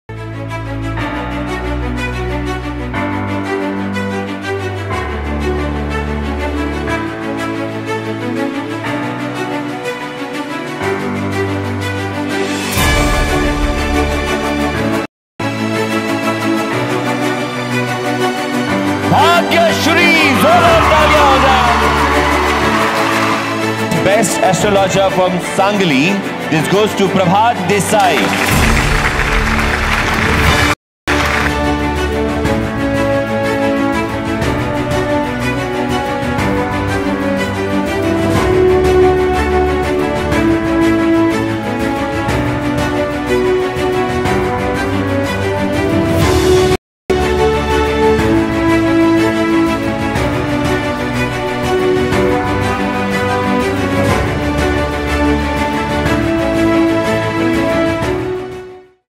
Best Astrologer in Sangli - Prabhat Desai (Awardee 2024)
Congratulations to Prabhat Desai for being honored as the Best Astrologer in Sangli at the prestigious Super Hero Awards 2024! Actress Bhagyashree presented him with this esteemed award on 10th March at the Award Ceremony Night organized by Forever Star India.

Prabhat Desai is renowned for his accurate predictions and insightful astrology consultations. His dedication and expertise have made him a trusted name in the field of astrology. Join us in celebrating his remarkable achievement!

🌟 About Super Hero Awards 2024: The Super Hero Awards recognize and honor individuals who have made outstanding contributions to society. Organized by Forever Star India, this event celebrates real-life heroes who inspire us all.

Join us as we delve into the realm of celestial wisdom with Prabhat Desai, whose remarkable talents and remarkable achievements continue to inspire and uplift all those who seek his guidance. 🌠